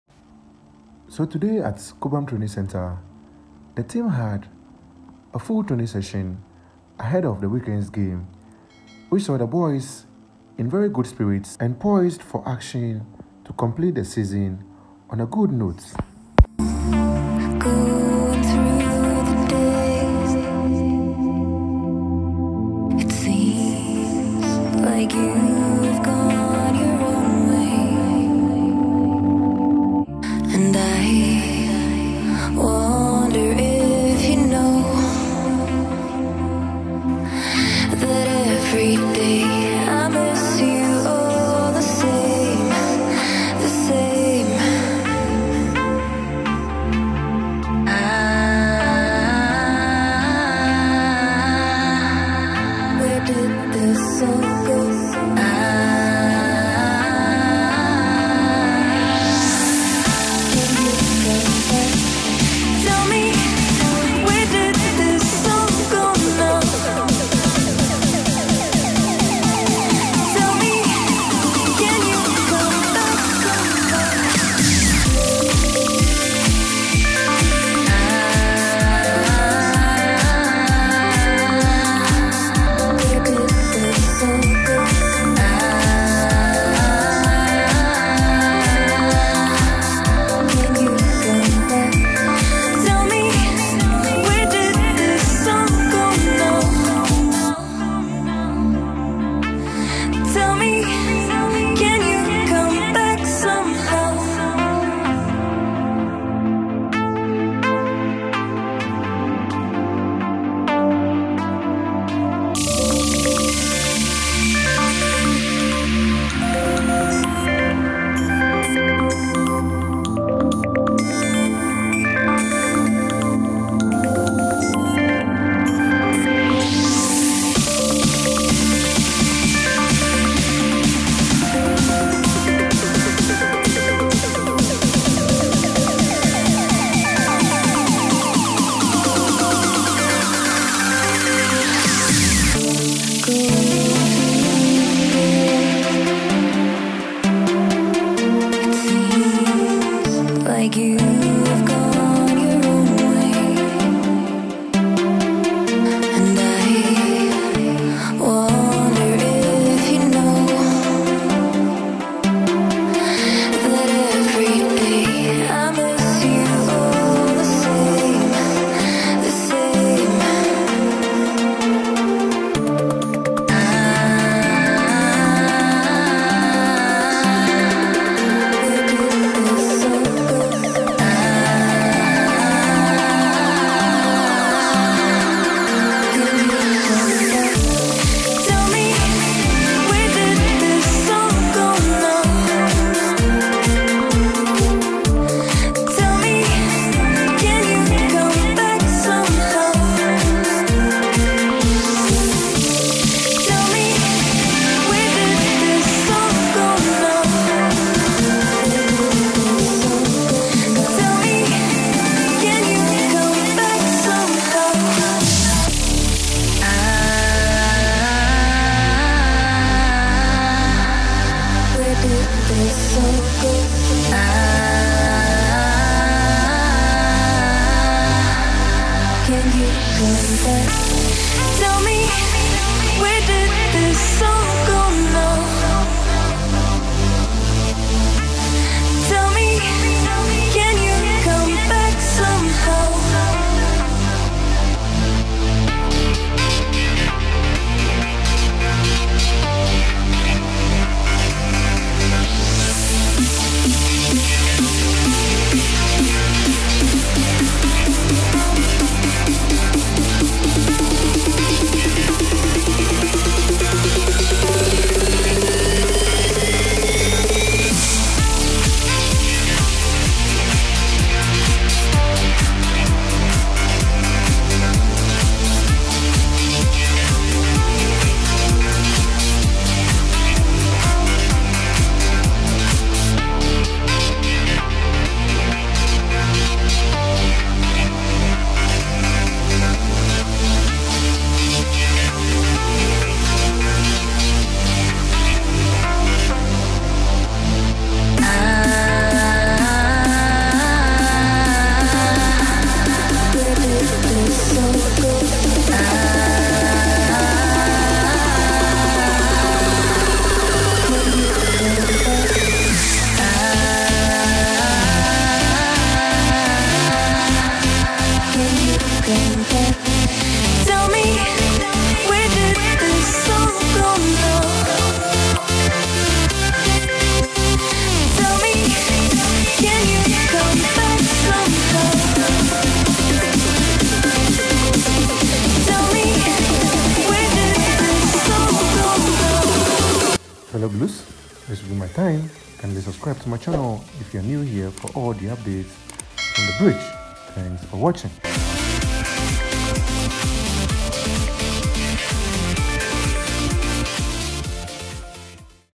✅Chelsea Players Full Training Session at Cobham Training Centre🔥Chelsea vs Wolves|Mount,Timo,Kai
Full Training session at Cobham!